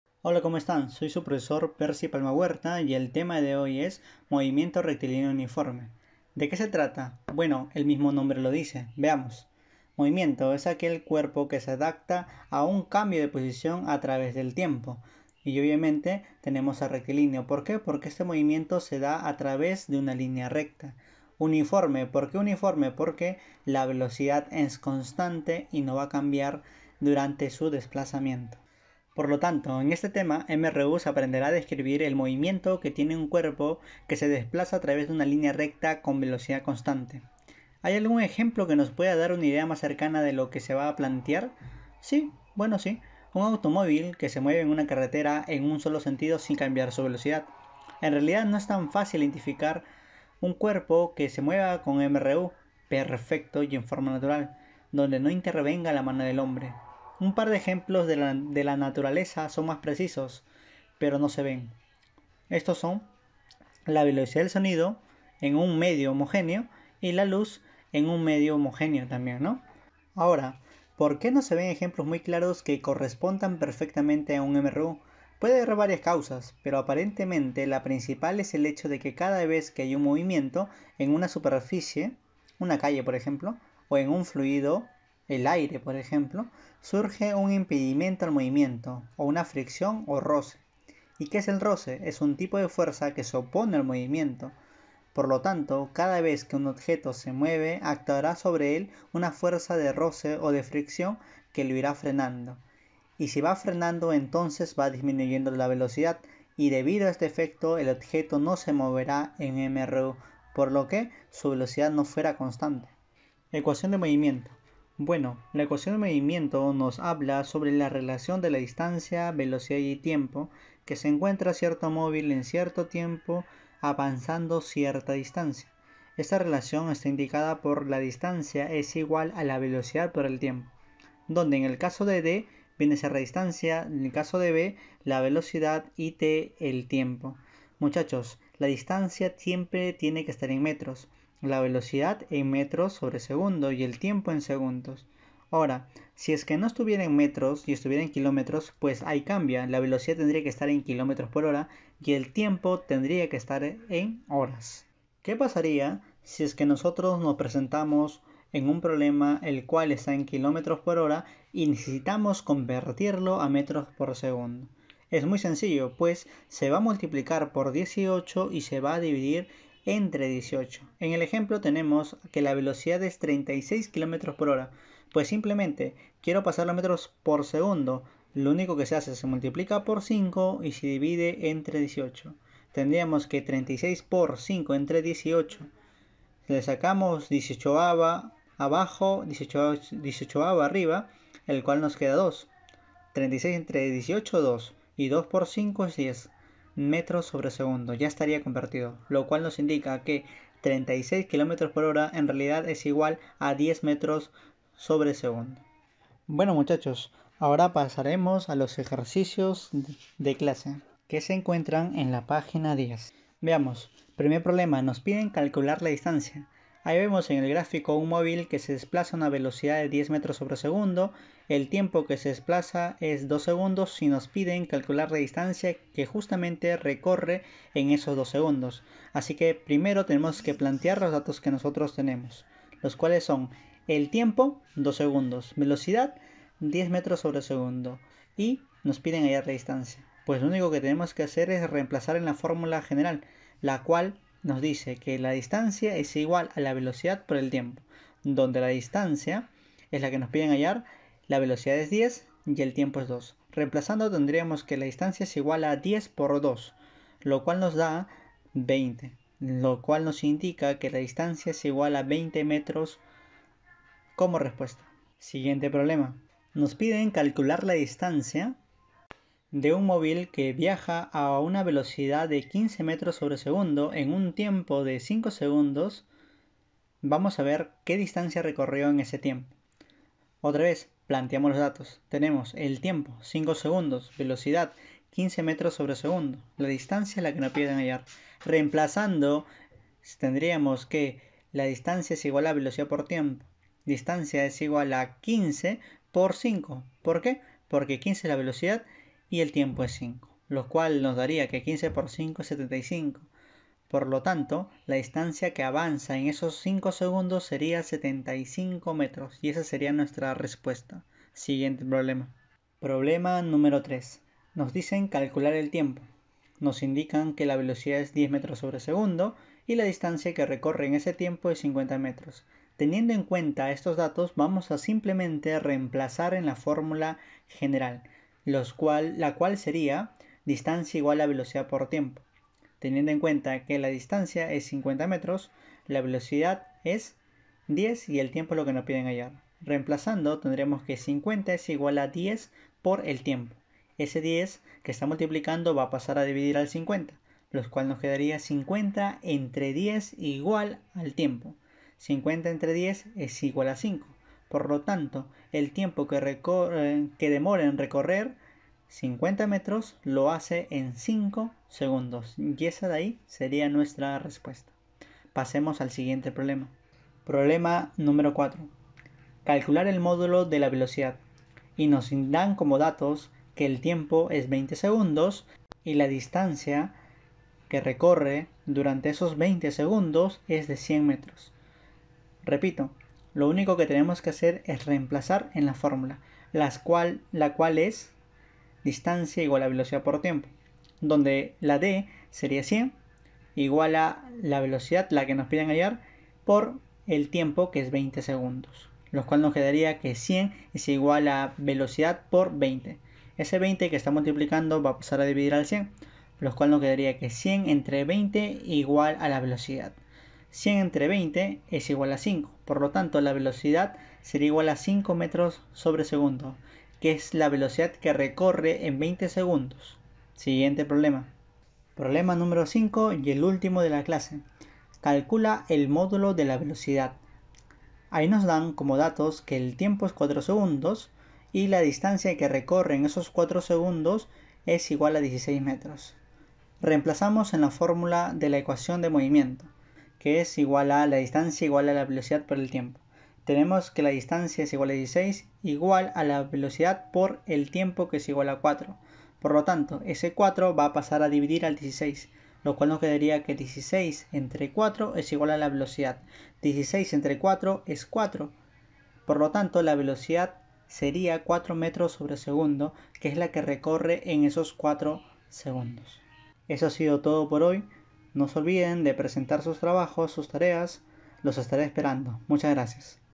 FÍSICA JUNIOR tema 2
TEMA 2: Movimiento Rectilíneo Uniforme (MRU)
no se olviden de resolver la tarea!!!